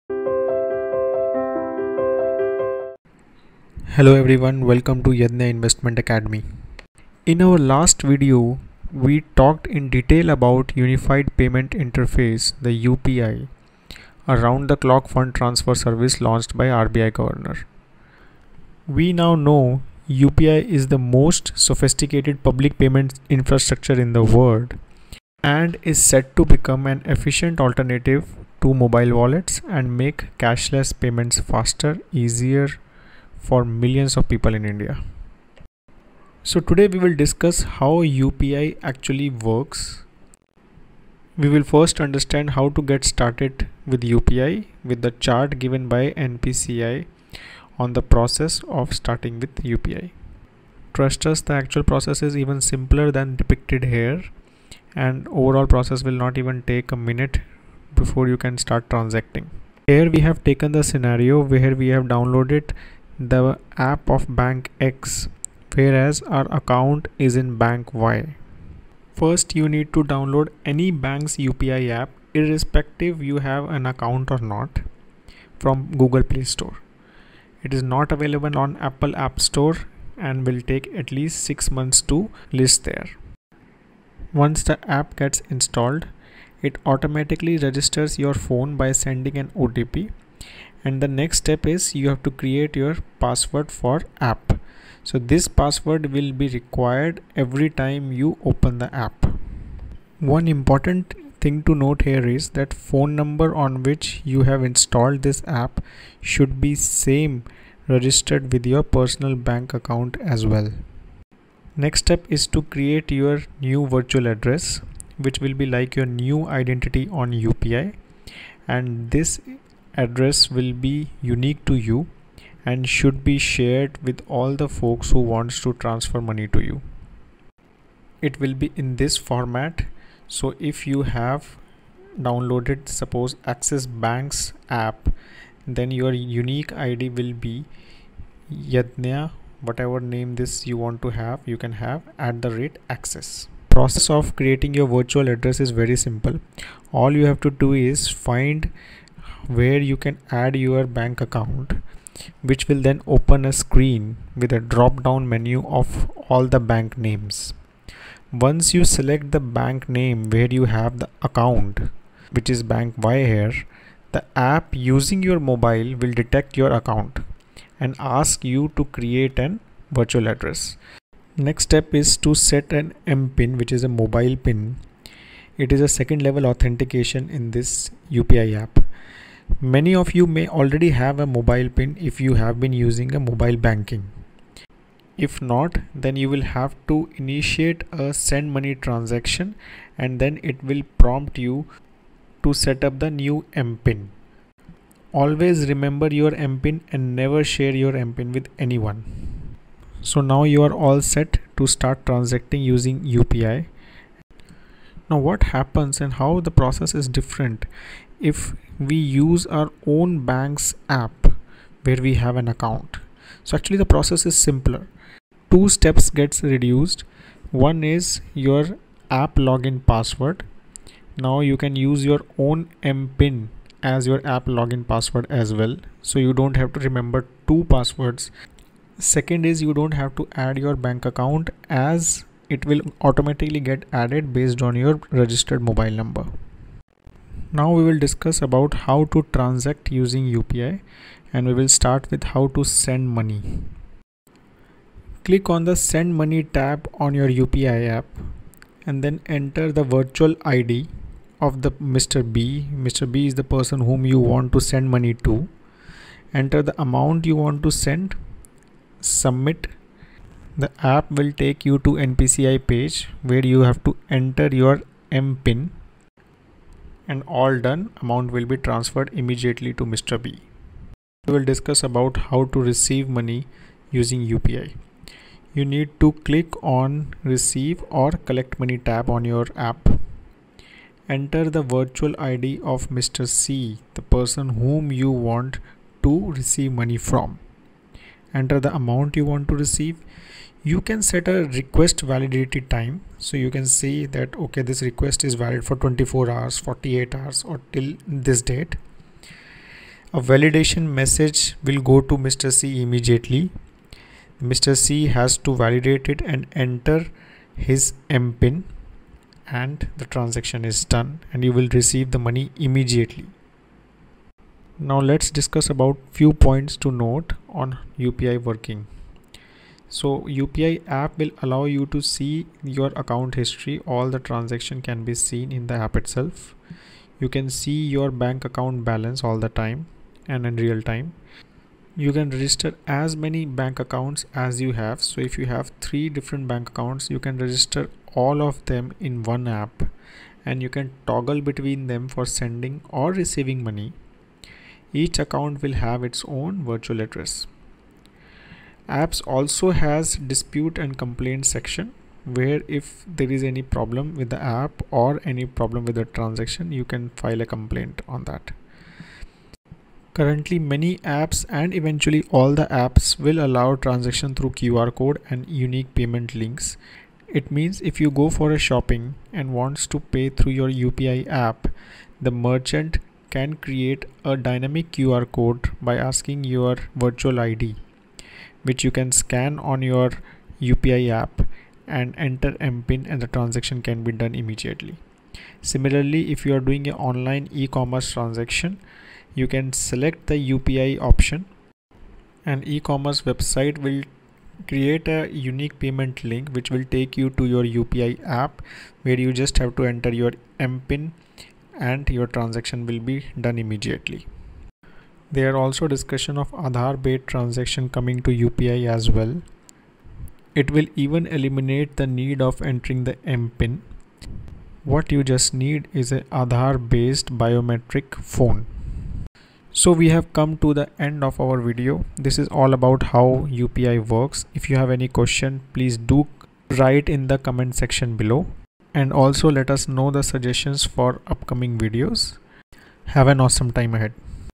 How UPI works? | How to get started with UPI?
India moved a step closer towards becoming a cashless economy with the launch of National Payments Corporation of India’s Unified Payment Interface (UPI). With UPI, will we finally see the tide turn away from cash towards digital money? There are many reasons to hope for this, as digital systems can be used to reduce the opacity of cash, and to build a person’s financial history, which will be an important tool for financial inclusion.

So how to get started? Some banks have integrated UPI into their existing apps. Some have launched new ones for it. For details about starting with UPI and transacting with UPI, please watch the video.